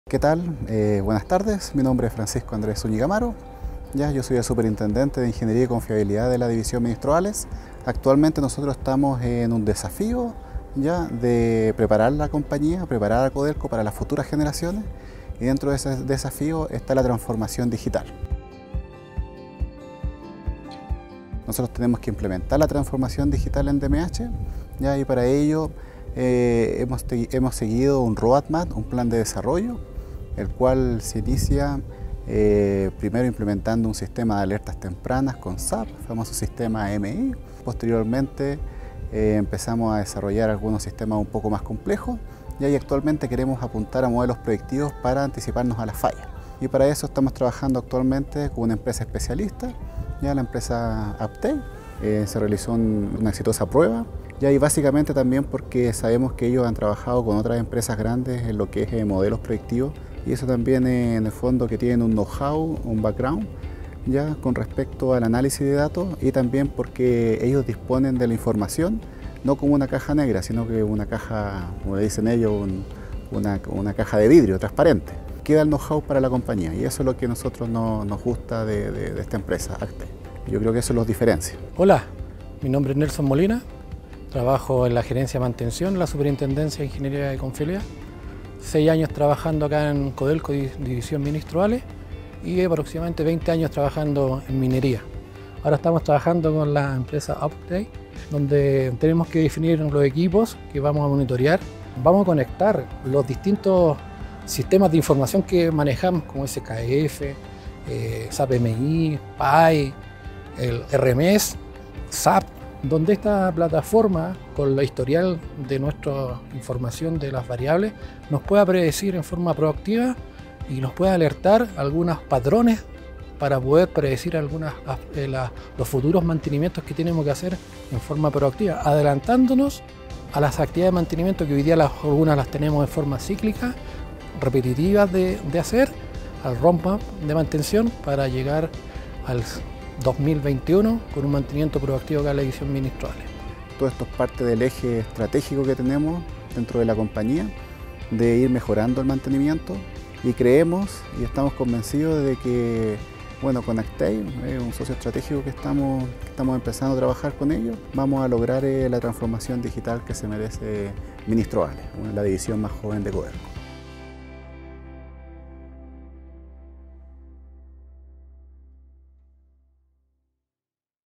Uptake + CODELCO
Codelco, the world’s largest copper producer, is working with Uptake to monitor the health of its mining equipment, ensure its operations run efficiently and predict maintenance needs ahead of time.